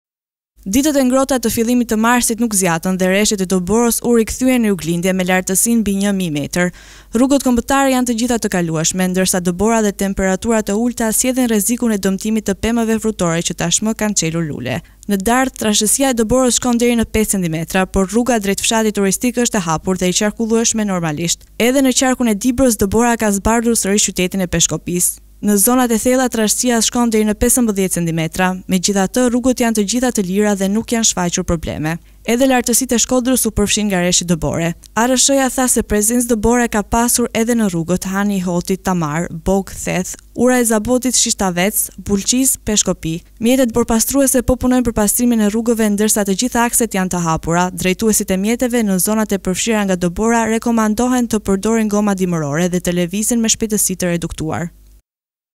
Dëborë në pranverë, rrugët e hapura në fshatrat turistike në Theth, Bogë dhe Dardhë
Dëbora u rikthye në ditën e 5 të pranverës. Mëngjesi i kësaj të marte i ka gjetur të zbardhur nga bora fshatrat turistikë në veri të vendit, Theth, Bogë dhe Tamarë, Vermosh.